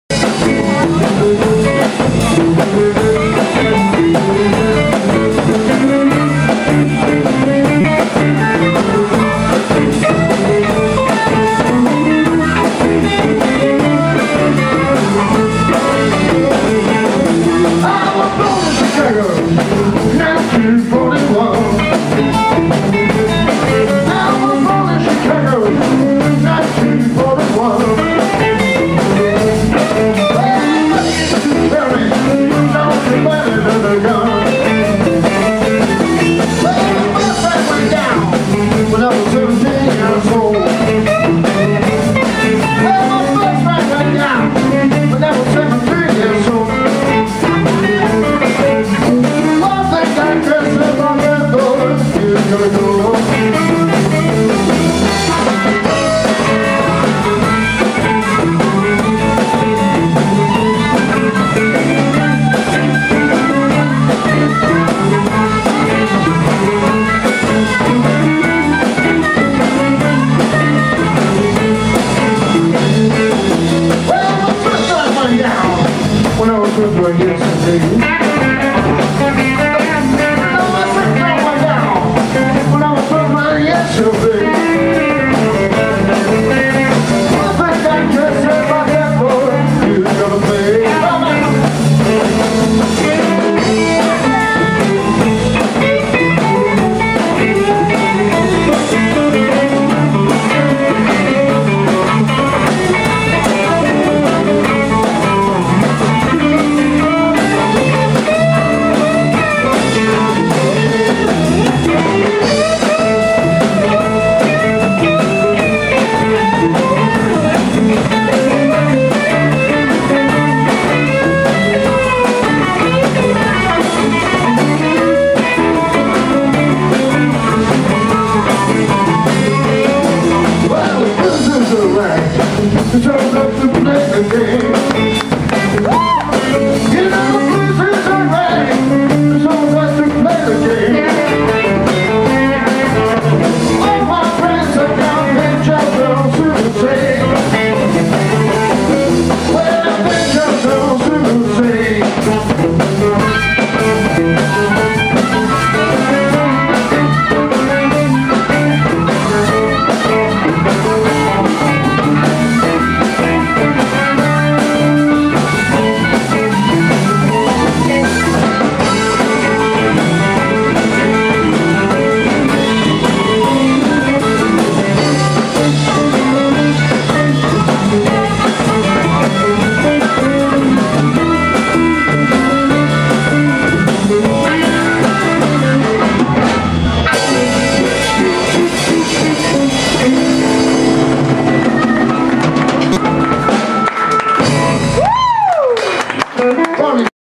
IBR jam feat. Beppe Maraschi & Andrea Miotto @Giro di Vite 25.1.2013 006
Giro di Vite Culturalcaffè, Sirtori LC     by Adriano Siberna